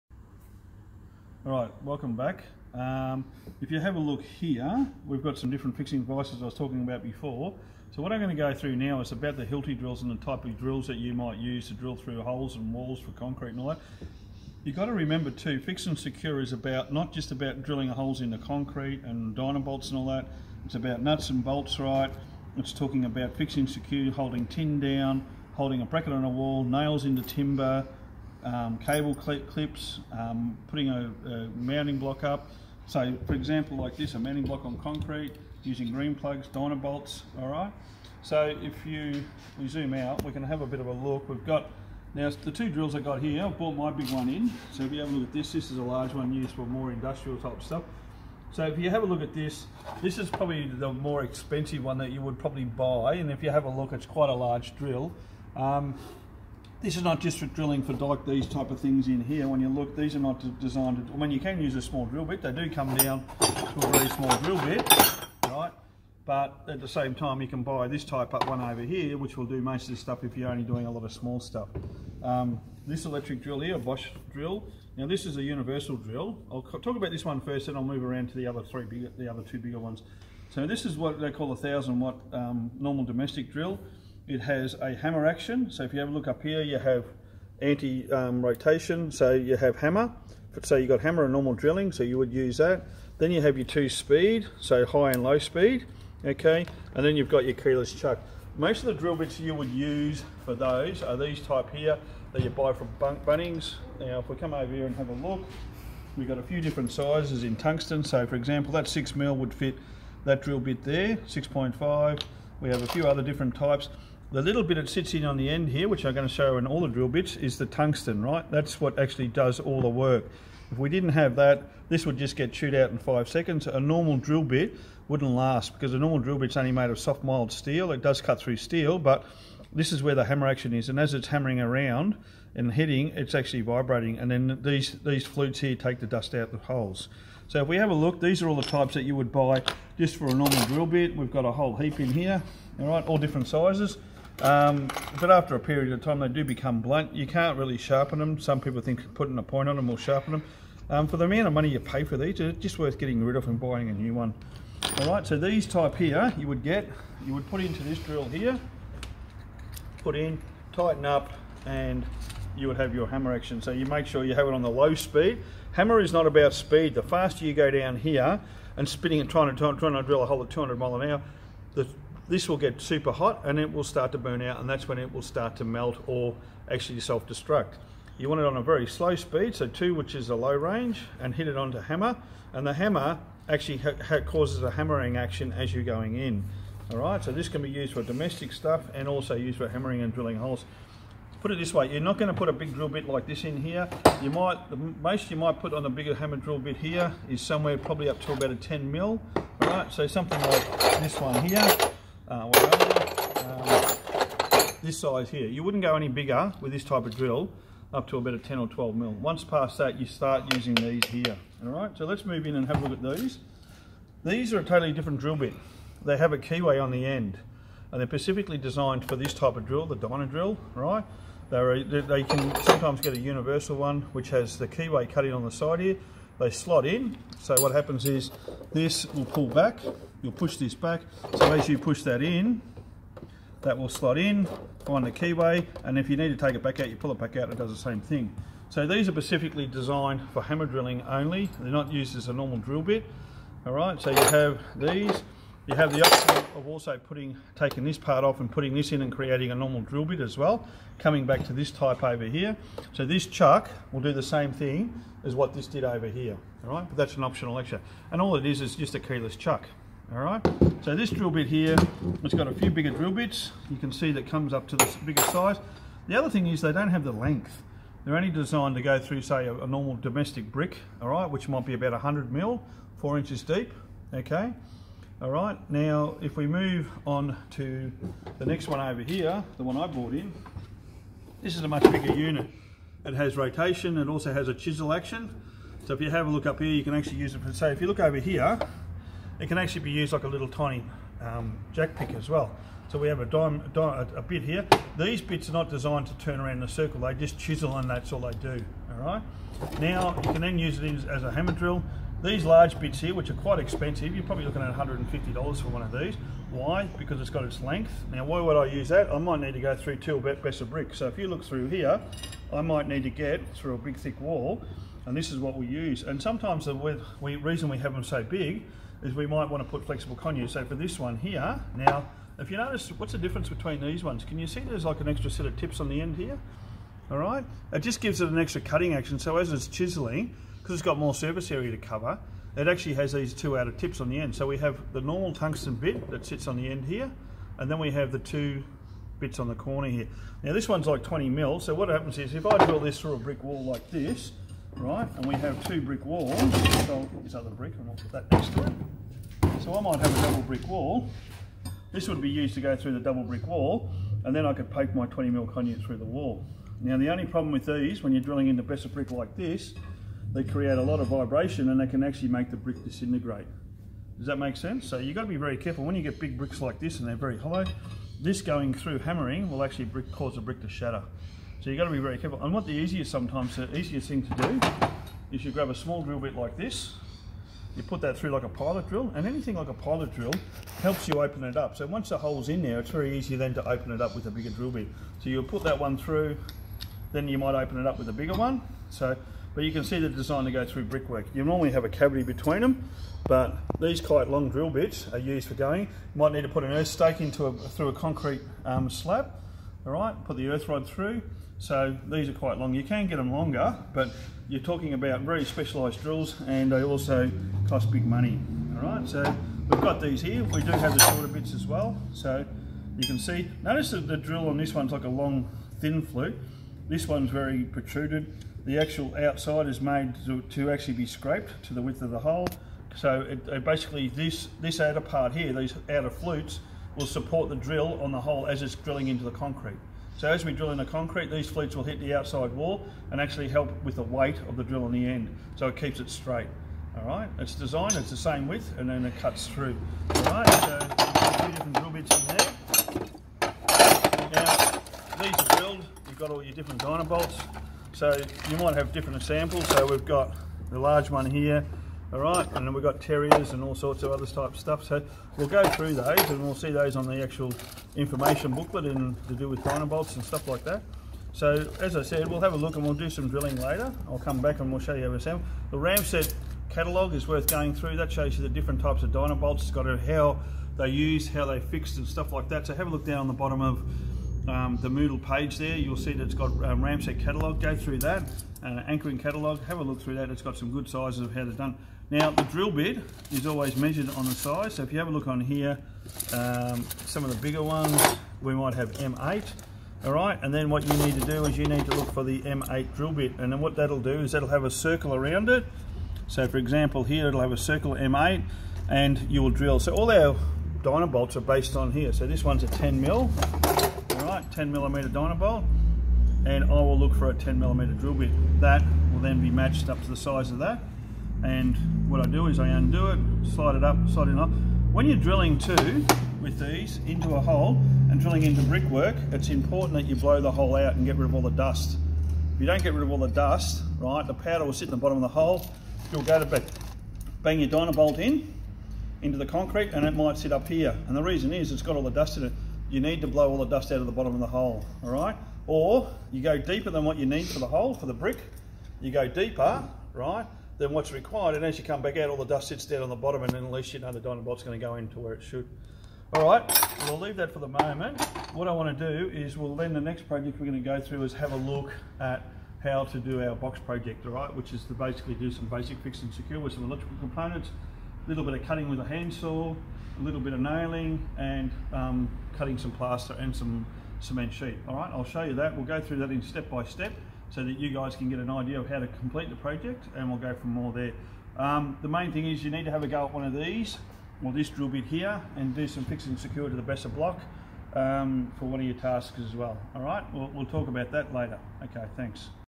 e105 explained about drills hammer and normal
How hammer drills work  and there uses